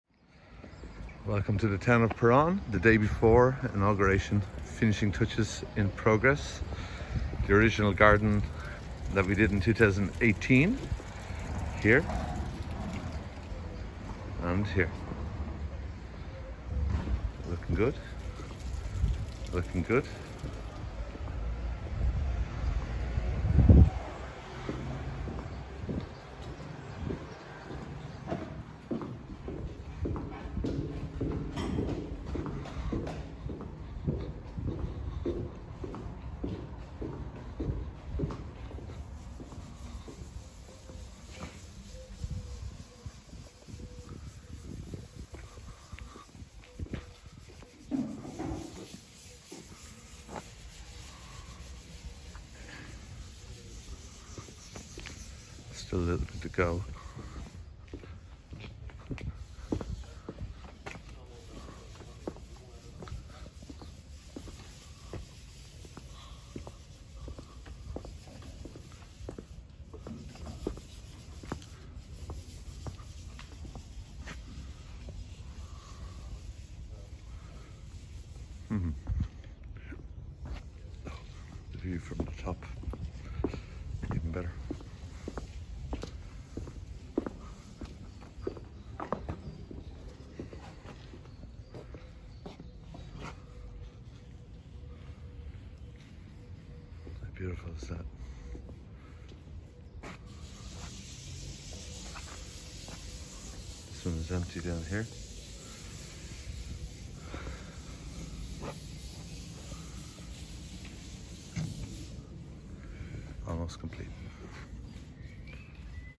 Jardin Philophrosyne Château de Péronne, France - Day Before Inauguration
Château de Péronne, France. 2 ans de conception et de réalisation, le Jardin Philophrosyne est terminé et inauguré - mon 2ème jardin conçu au château du 13ème siècle et le plus grand musée de la guerre d'Europe, la Historial de la Grande Guerre
Aux habitants de Péronne, à l'équipe de L'Historial et à vous qui avez souri à mes côtés - merci - votre accueil vous ressemble, et toujours magnifique.

Photos d'inauguration à suivre, dès qu'elles me parviendront.

Chateau de Peronne, France. 2 years of design and realization, Jardin Philophrosyne is complete and inaugurated - my 2nd garden designed at the 13th century castle and Europe largest war museum Historial de la Grande Guerre.
To the people of Péronne, the team at The Historial and you who smiled by my side - thank you - your welcome is like you, and always beautiful.